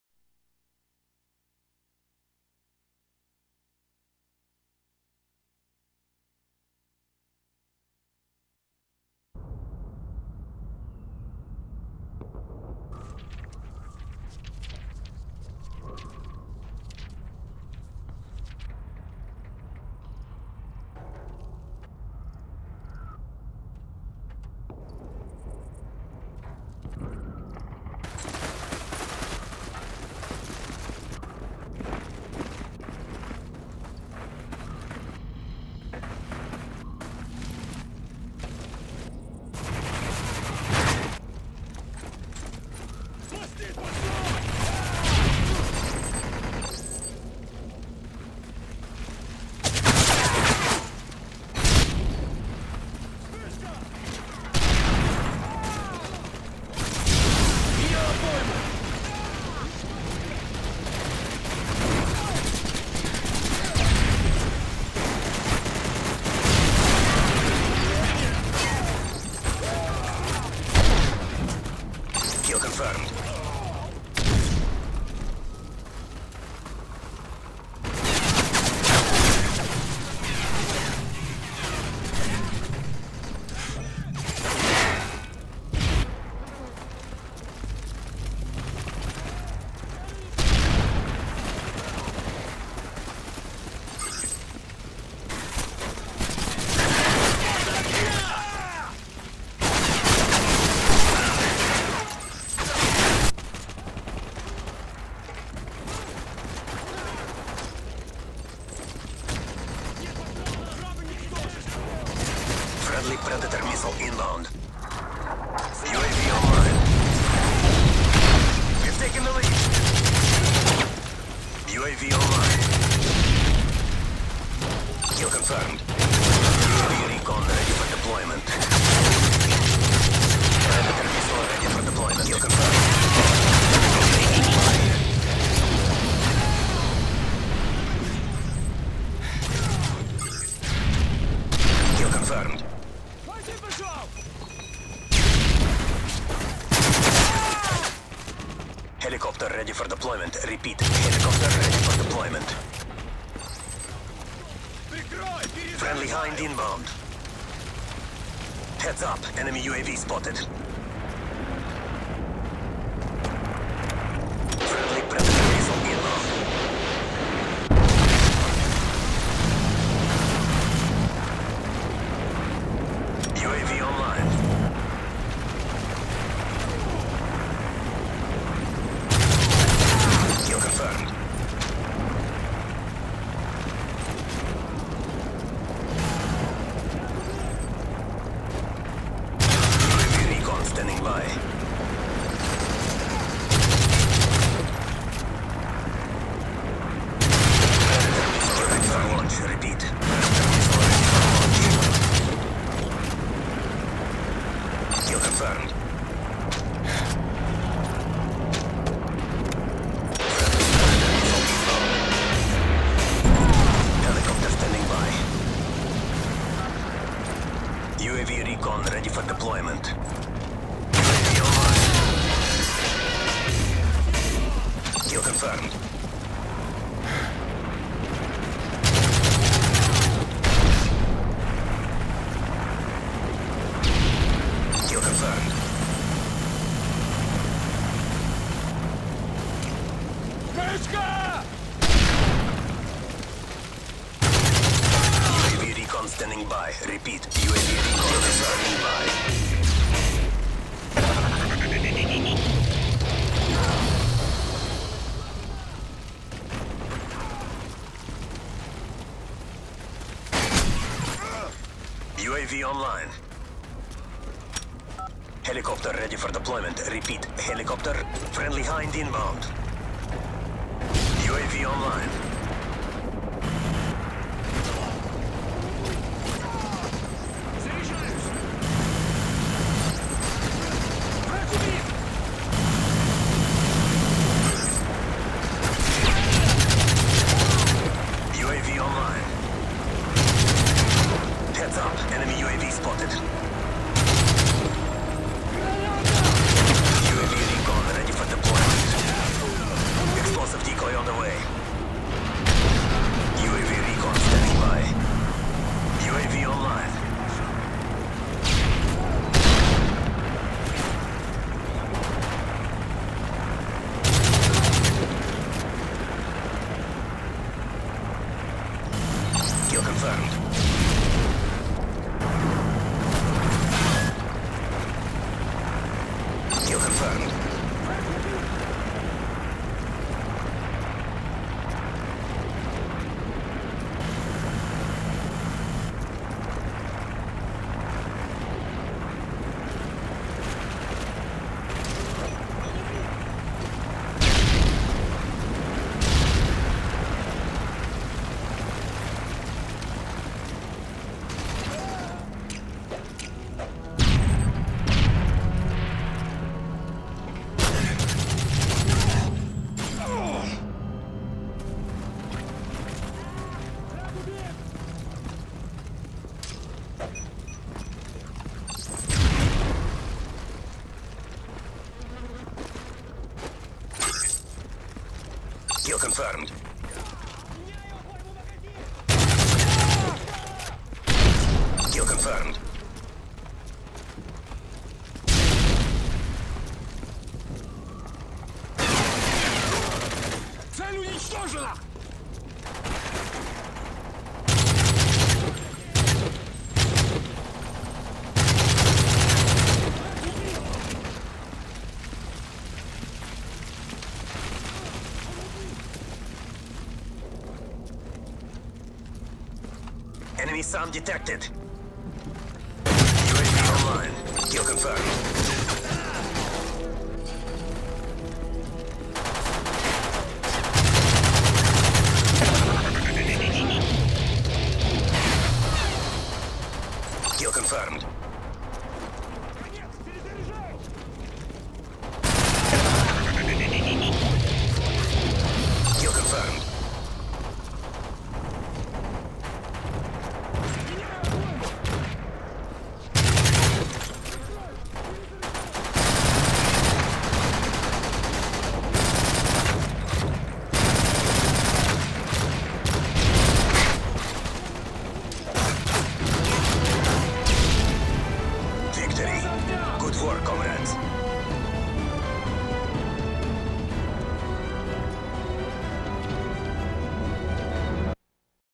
How To Make A Team Forfeit
Was playing kill confirmed and ended up making the other team rage quit.